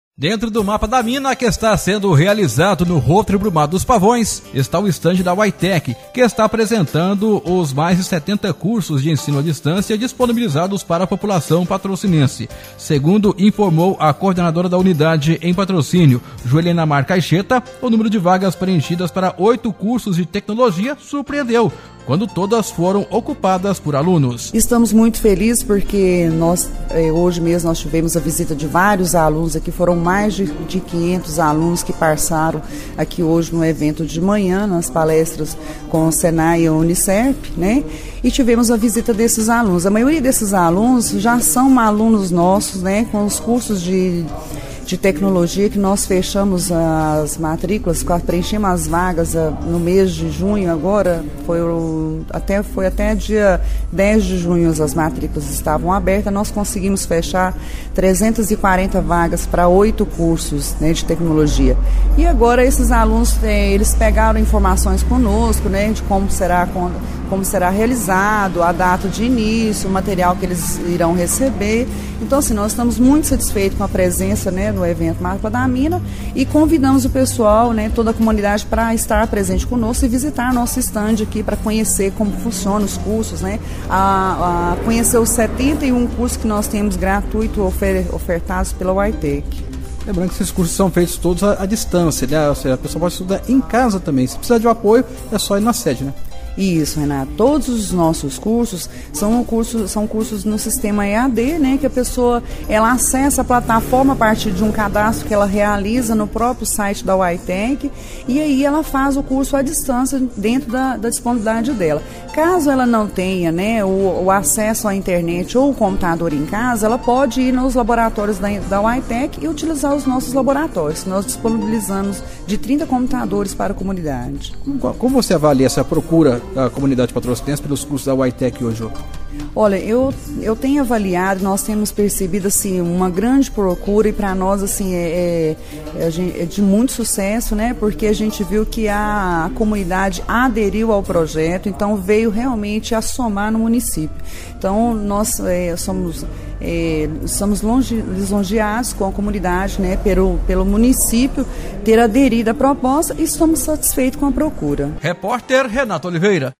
Uaitec tem recebido grande número de alunos em seus cursos EAD
UAITEC que está apresentando, em seu stand no Mapa da Mina, os vários cursos de ensino à distância disponibilizados para a população patrocinense.